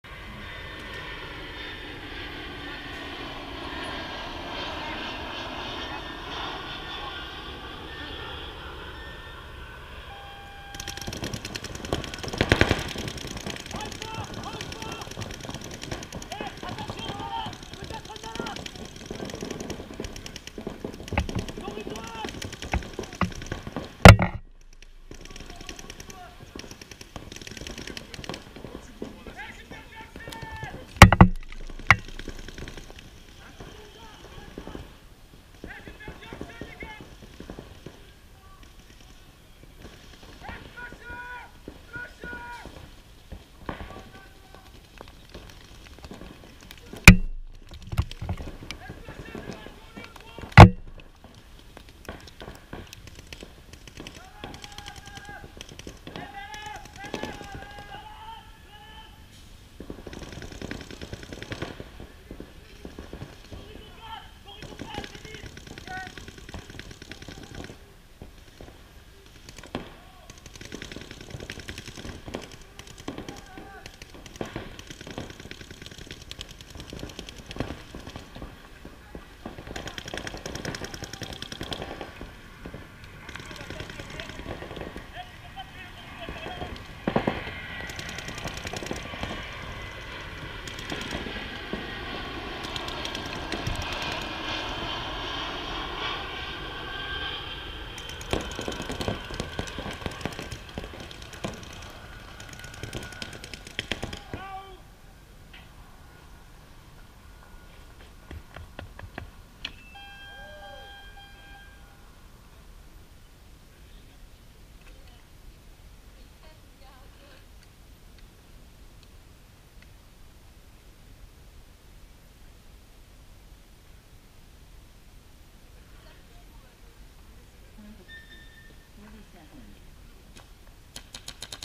CFE: Match 2: TonTon vs Triade
Championnat de Paintball Elite / 3ième Manche 2017 / Pbs 91
Format Semi 5 joueurs Jeu Long.
Triade Saulx les Chartreux vs TonTon Toulouse